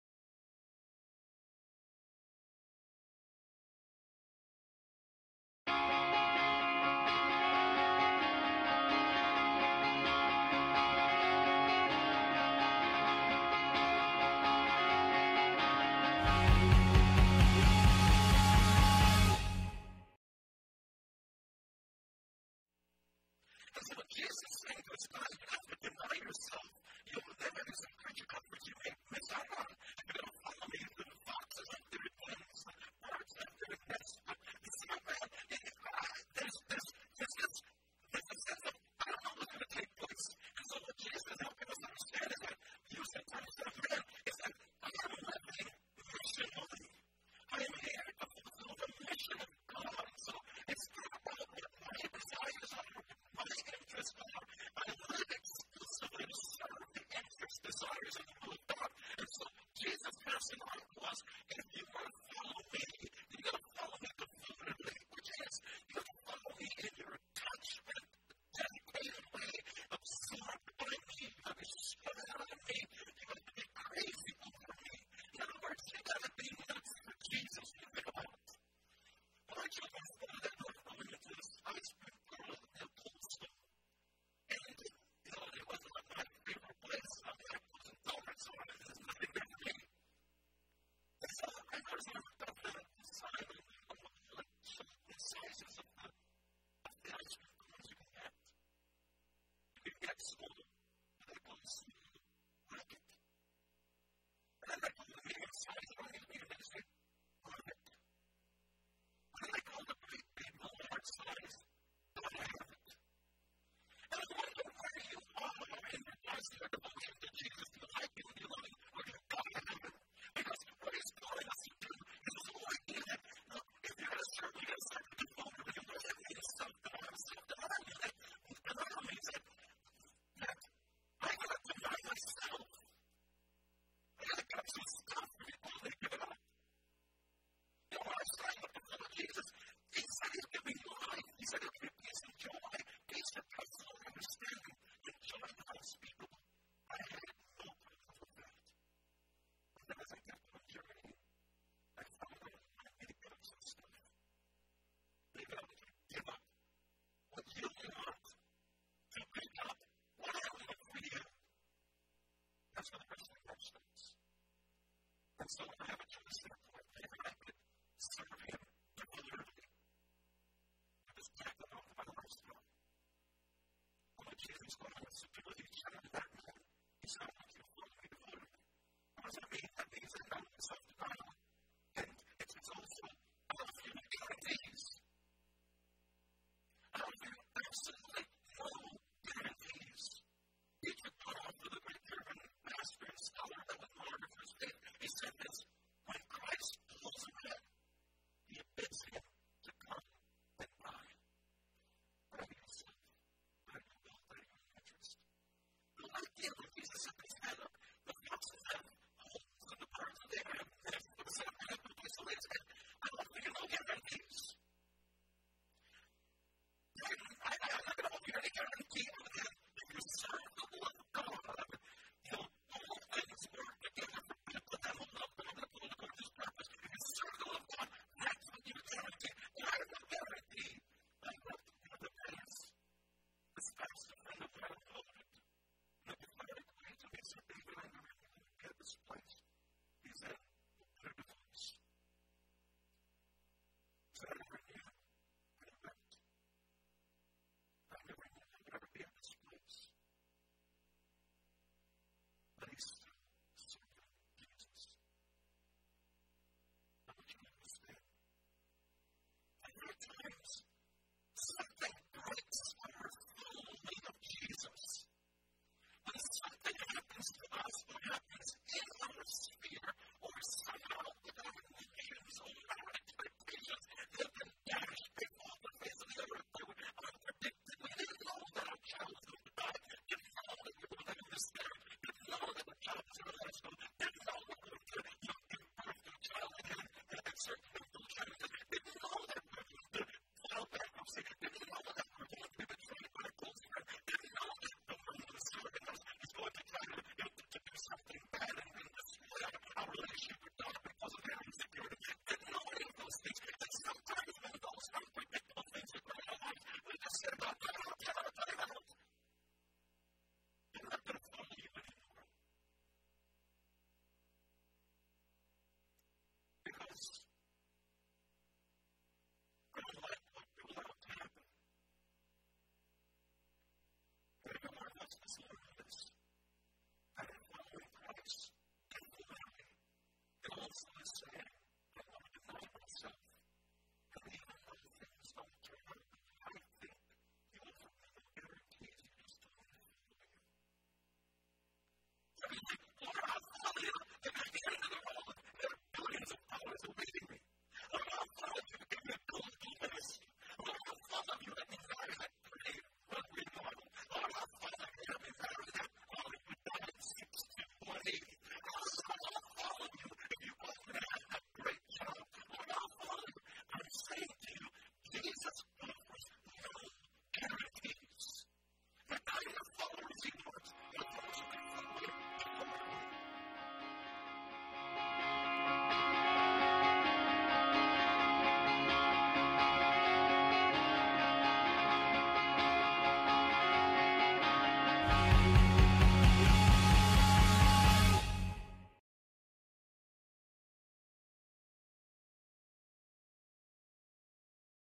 Devoted Followers - Jesus and His Followers - David D. Ireland, Ph.D.
This is a highlight moment from the sermon "Jesus and His Followers" by David D. Ireland, Ph.D.

This is a Christ Church production.